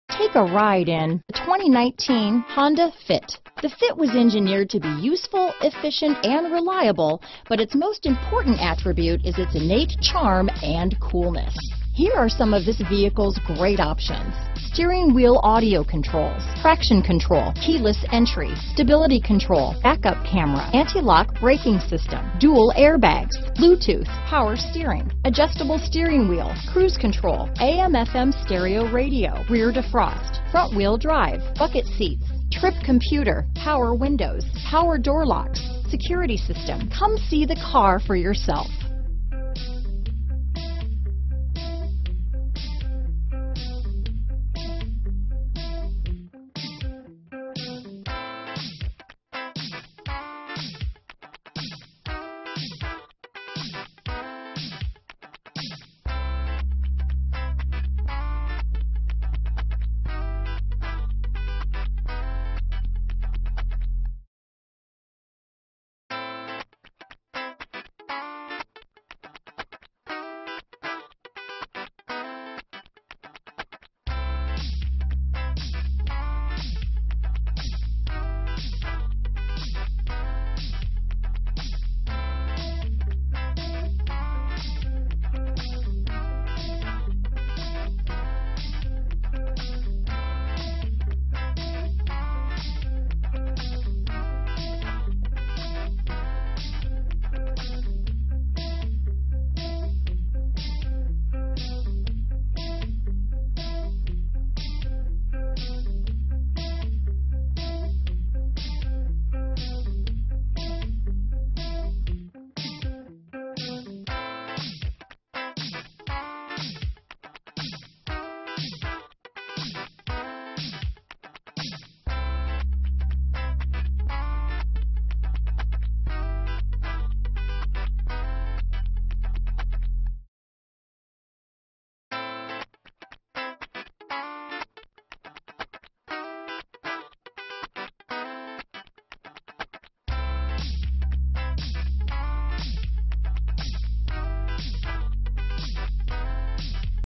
2019 Honda Fit Aurora, Denver, Highland Ranch, Parker, Centennial, CO 42591
Aegean Blue Metallic New 2019 Honda Fit available in Aurora, Colorado at

Scores 40 Highway MPG and 33 City MPG! This Honda Fit delivers a Regular Unleaded I-4 1.5 L/91 engine powering this Variable transmission. Wheels: 15 w/Full Covers, VSA Electronic Stability Control (ESC), Urethane Gear Shift Knob.* This Honda Fit Features the Following Options *Trip Computer, Transmission: Continuously Variable (CVT) -inc: ECON button and ECON mode indicator, Torsion Beam Rear Suspension w/Coil Springs, Tires: 185/60R15 84T AS, Tailgate/Rear Door Lock Included w/Power Door Locks, Strut Front Suspension w/Coil Springs, Steel Spare Wheel, Single Stainless Steel Exhaust, Side Impact Beams, Seats w/Cloth Back Material.* Stop By Today *Test drive this must-see, must-drive, must-own beauty today at Mile High Honda, 2777 S Havana St, Aurora, CO 80014.